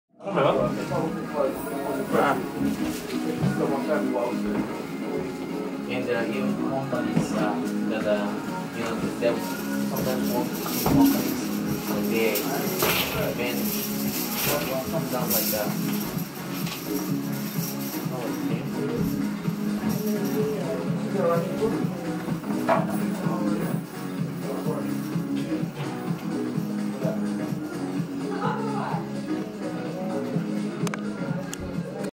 Prostitution Museum in Amsterdam
Taking a prostitution quiz at the museum in Amsterdam...